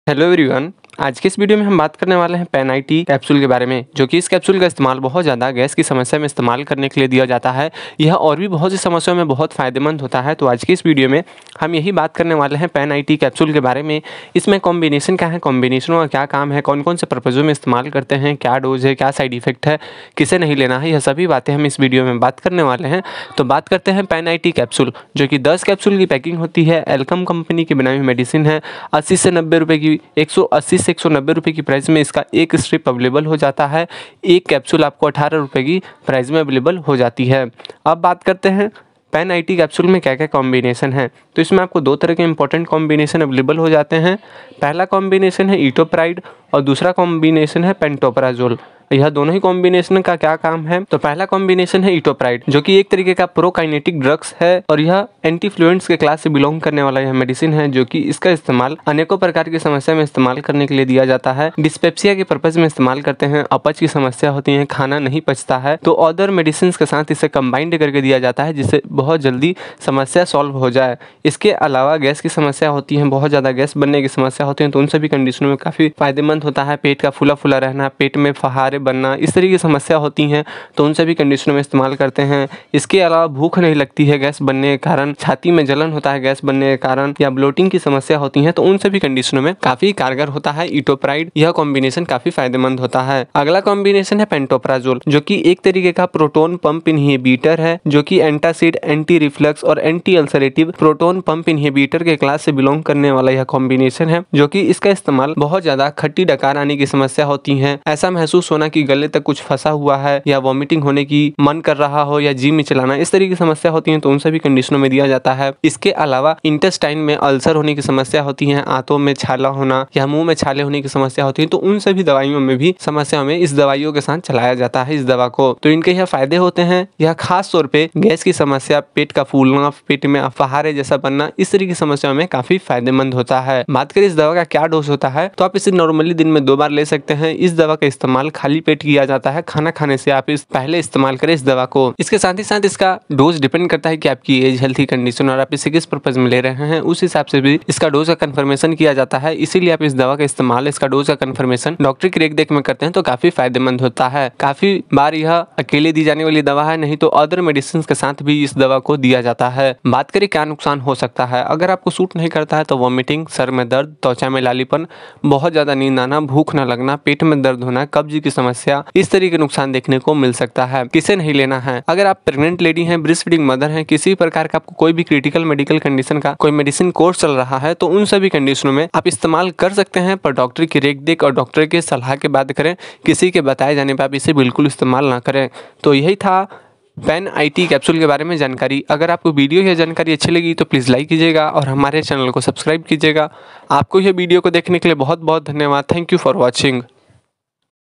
गैस,अपच,पेट फूलना कुछ मिनट में खत्म करे,Pan It Capsule Uses + All Details,Pantoprazole,Itopride
Hello Everyone Aaj Ke Is Video Me Hm Baat Krne Wale Hai Pan It Capsule Ke Bare Me Iske Fayde Aur Nuksan Aur Kaise Istemaal Kre Yh Sabhi Batte Is Video Me Hm Baat Krne Wale Hai

All Topics Cover In This Video About Pan It Capsule

pan it capsule use
pan it capsule side effects
pan it capsule benifit
pan it capsule dose
pan it capsule compositions